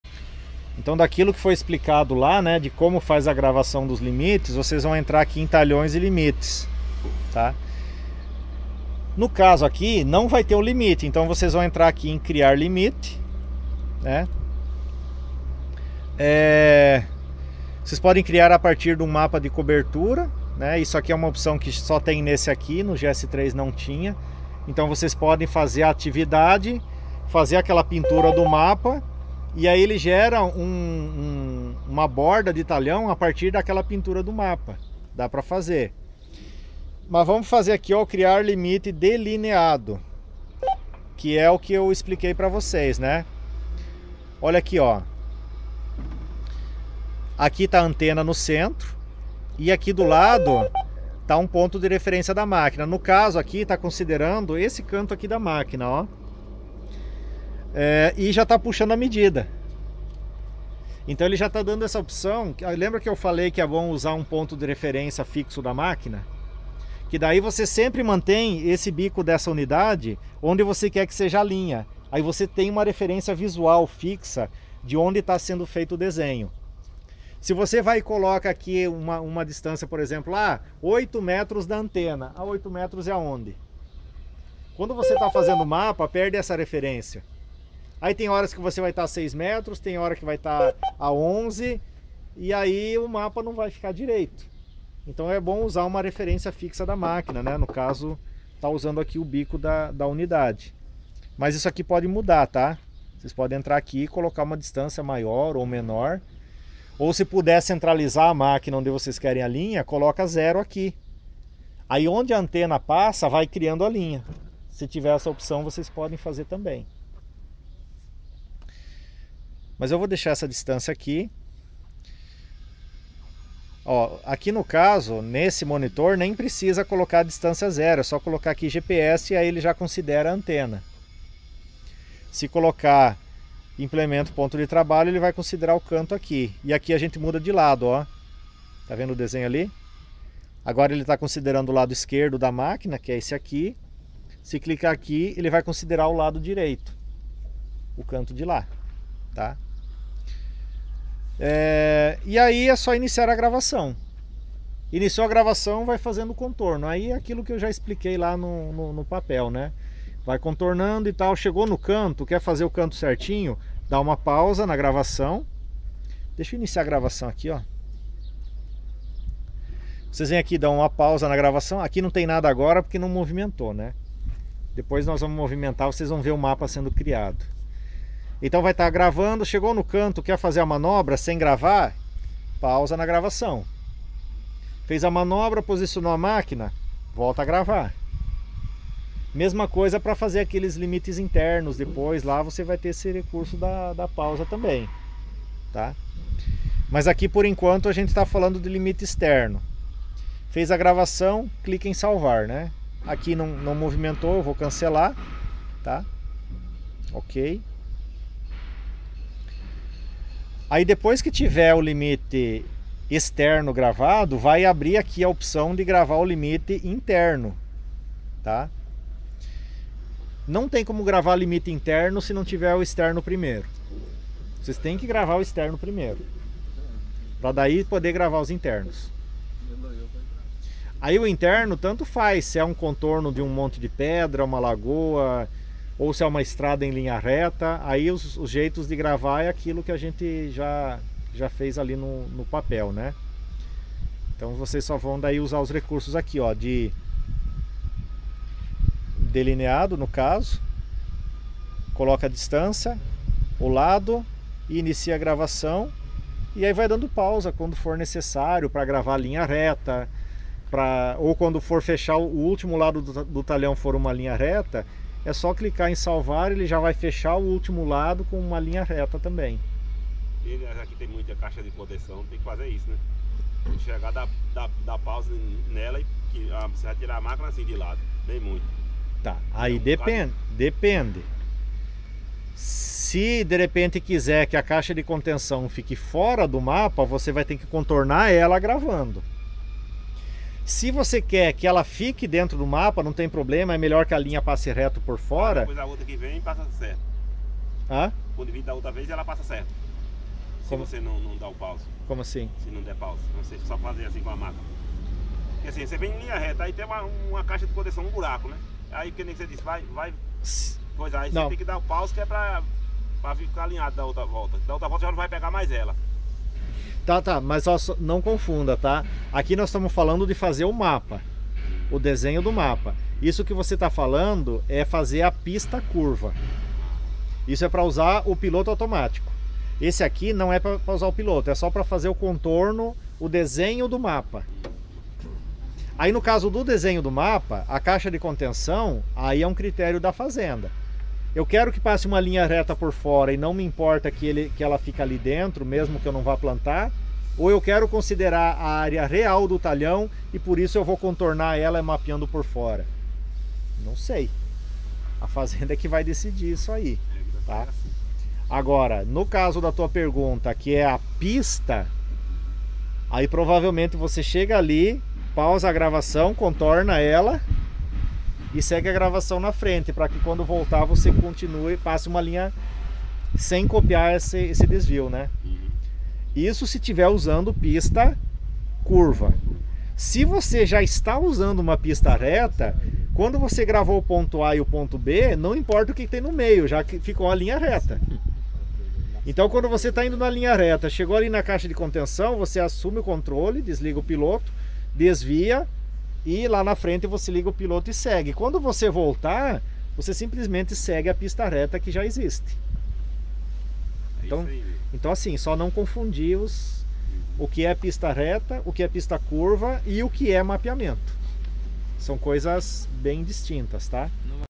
Como criar limites no monitor John Deere GS4?
Veja como configurar o monitor para fazer a gravação dos limites de talhões no Monitor John Deere GS4.